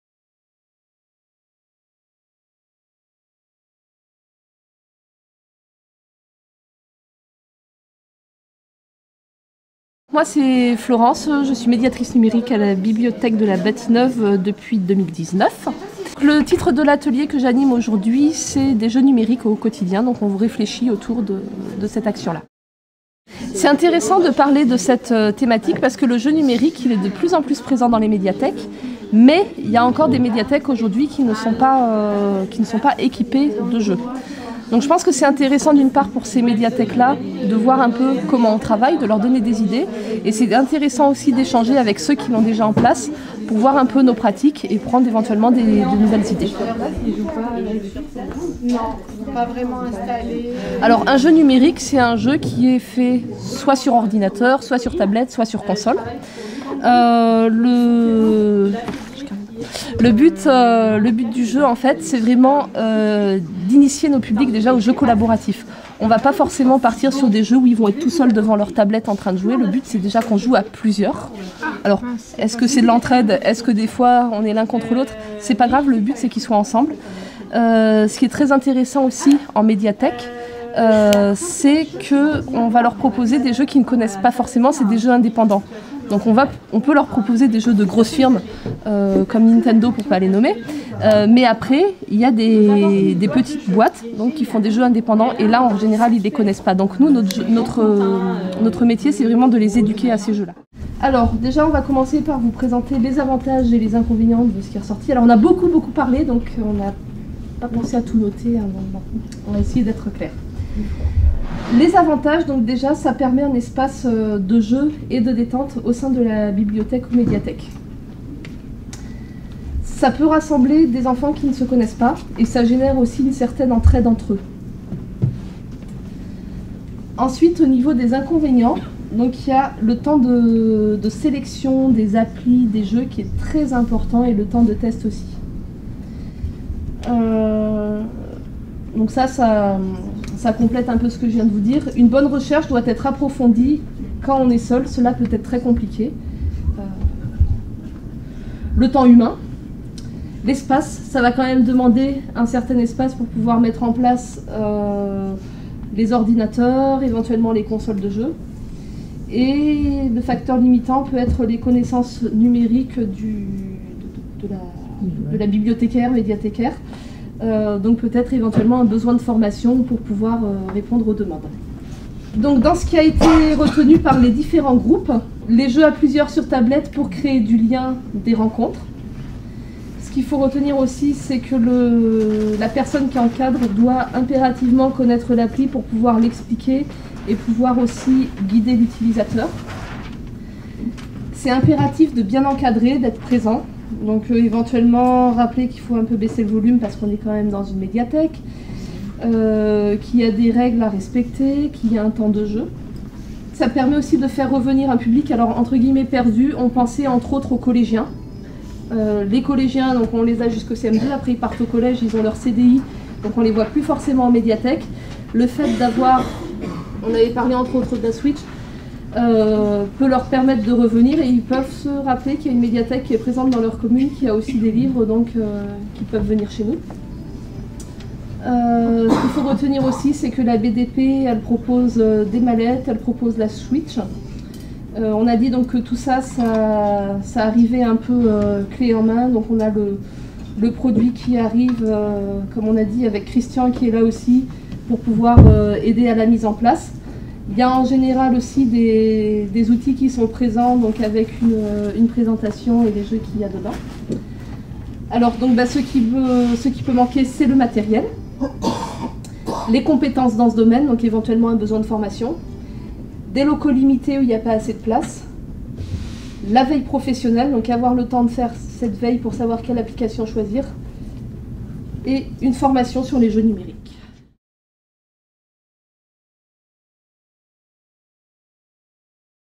Des jeux numériques au quotidien,
Par Florence, Médiathèque de la Bâtie-Neuve, dans le cadre de la 17e Rencontre des bibliothécaires, le Jeu en bibliothèque : un atout maître.